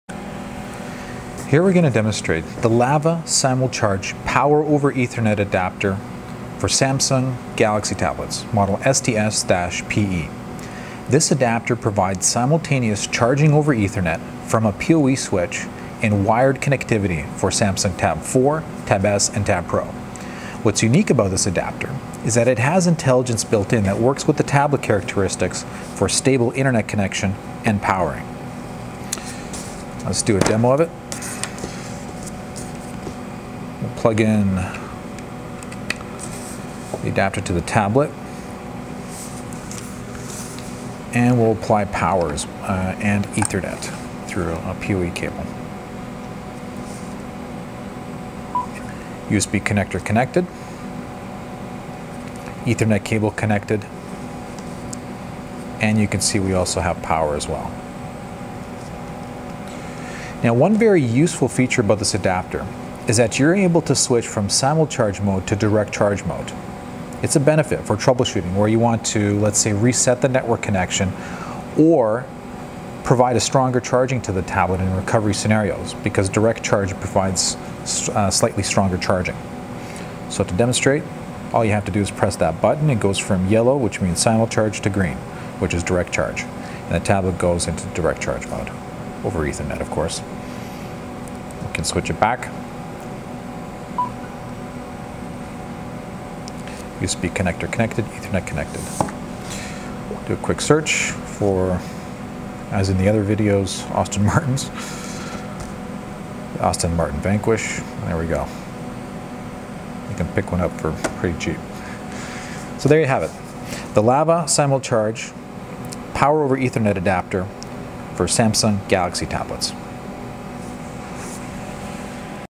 Demo of PoE for Samsung Galaxy Tab A/E/S2 with LAVA nSTS-PE Power over Ethernet Adapter
LAVA SimulCharge nSTS-PE Ethernet Adapter with PoE for Samsung Galaxy Tab, provides Power over Ethernet, LAN connectivity, and the ability to use the tablet in USB OTG/Host mode while being powered.

Compatible with Galaxy Tab A (8.0", 10.1") Tab S2 ( 10.5") Tab E (8.0", 9.6")